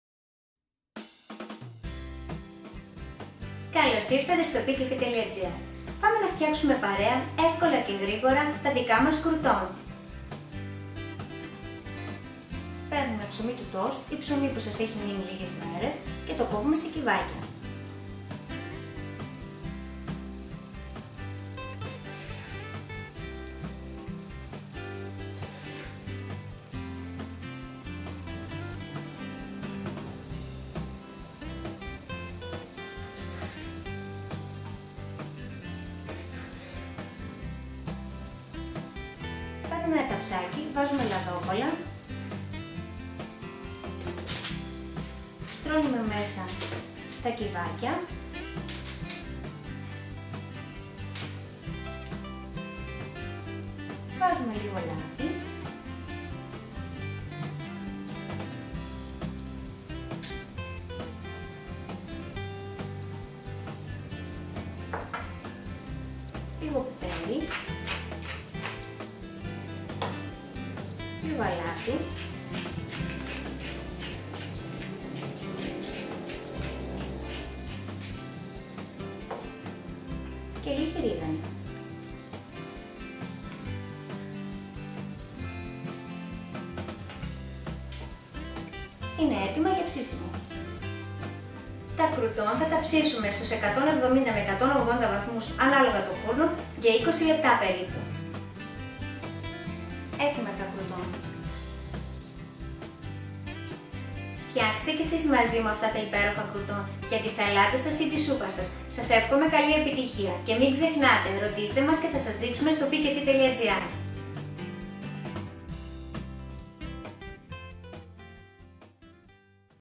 Κρουτόν - Krouton - StoPikaiFi.gr
Φτιάξτε μαζί μας σπιτικά κρουτόν StoPikaiFi.gr

Προετοιμασία :  10 λεπτά
Ψήσιμο: 25 λεπτά

Υλικά:
Ψωμί κανονικό η του τοστ
2 -3 κουταλιές της σούπας λάδι
1 κουταλάκι του γλυκού ρίγανη
Μια πρέζα πιπέρι
1 κουταλάκι του γλυκού αλάτι